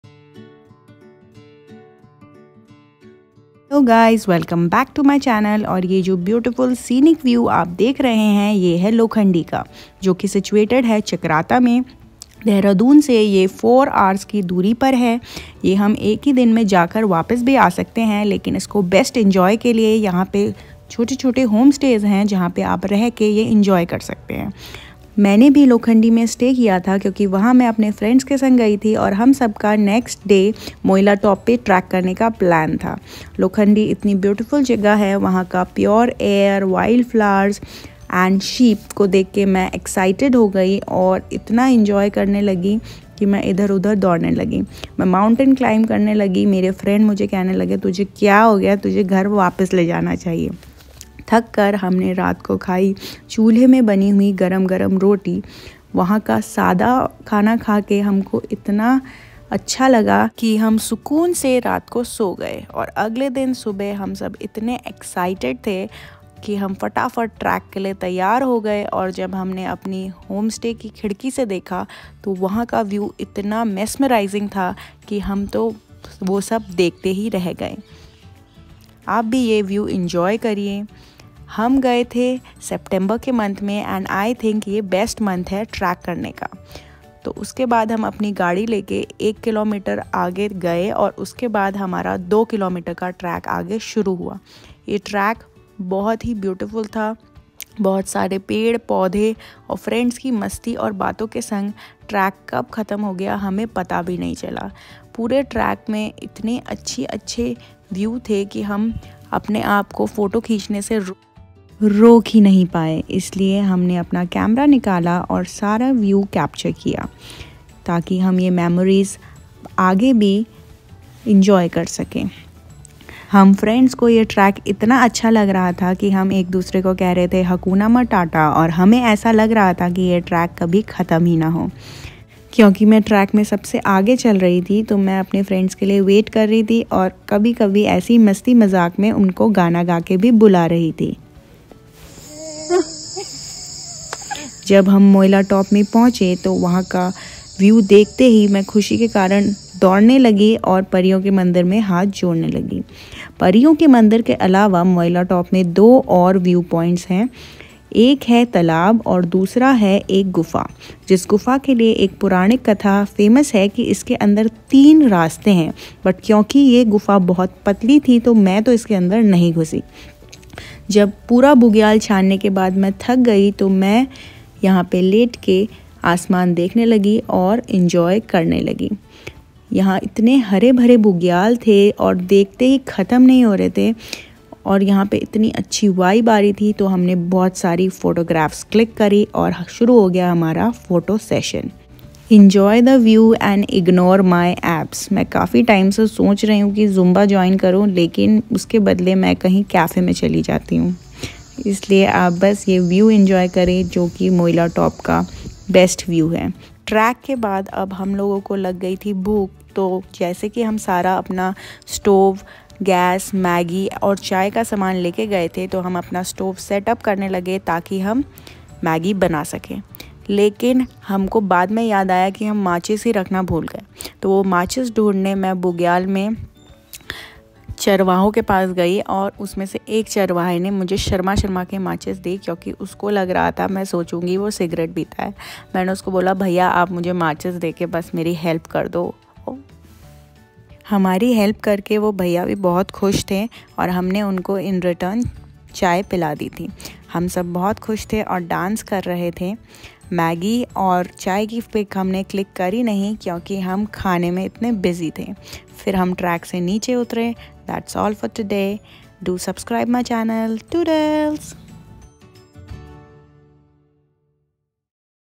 🧚 pariyo ka mandir|| Moila top|| Lokandi||Chakrata
Moila top is a 2 km trek from Lokandi in Chakrata. For any queries regarding the trip, feel free to dm me👍travel